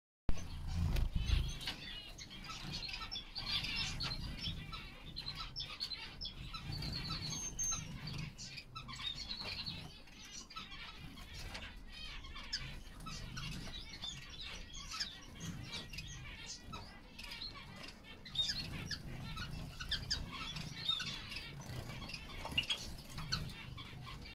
Magpie mannikin (Lonchura frigilloides) & Java sparrow (Lonchura oryzivora).
A selection of munia species living alongside one another in Chairmans collection.